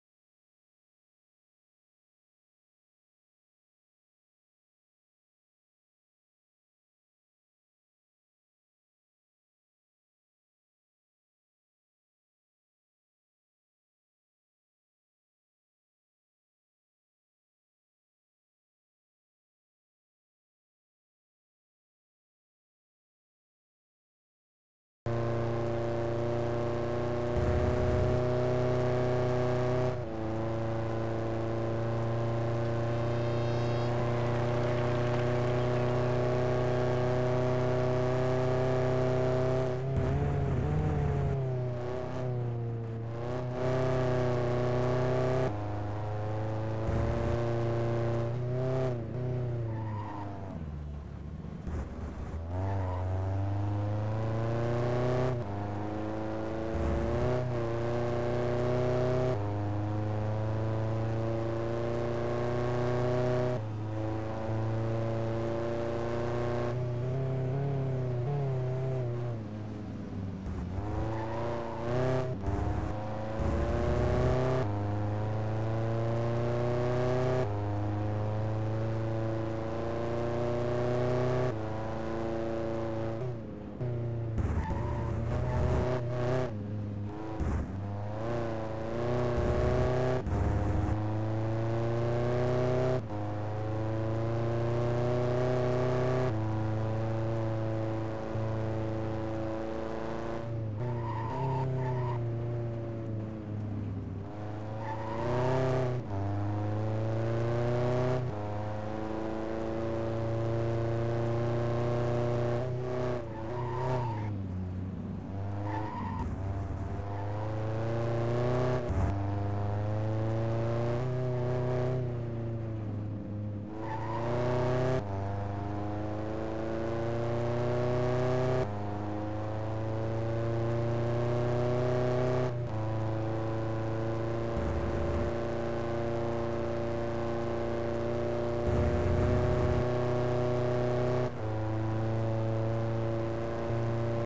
VSR HTCC 2011 Algarve pole lap
VSR HTCC '11 bajnokságának 4. fordulójában, Algarve-ban futott pole köre.

Versenyző: #14 - Kovács Gábor
Autó: SEAT León TDI '08 / privát
Kör: 1:51.681

The pole lap from Algarve, which was the 4th round of VSR HTCC '11 series.

Driver: #14 - Gábor Kovács
Car: SEAT León TDI '08/ Private
Lap: 1:51.681